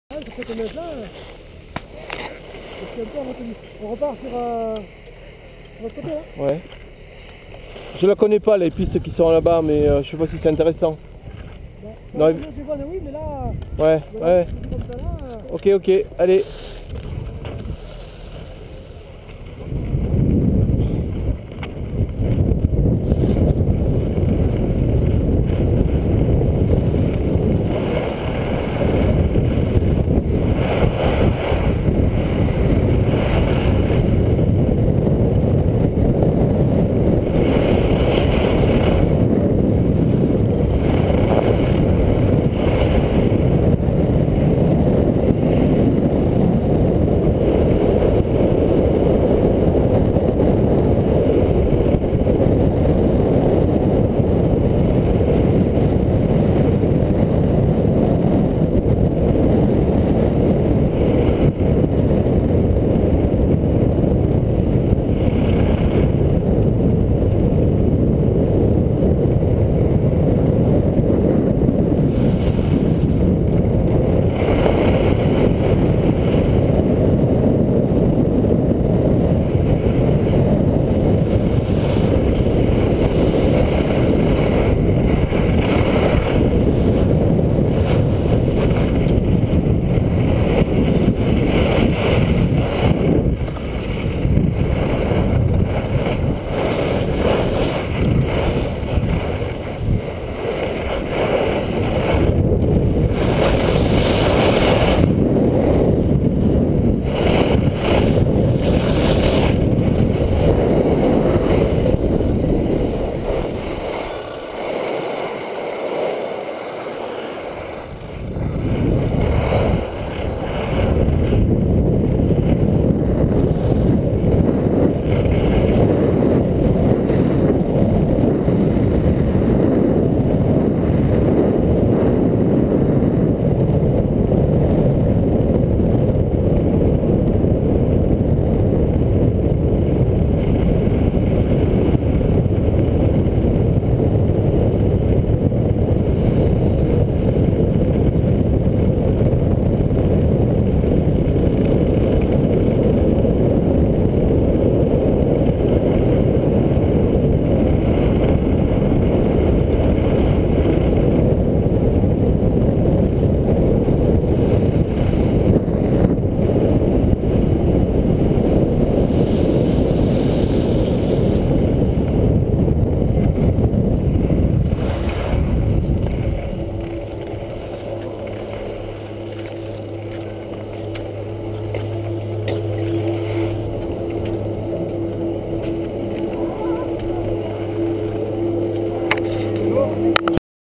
Ski assis (handiski) à La Mongie/Barèges, sur Caoubère, février 2012
Handiski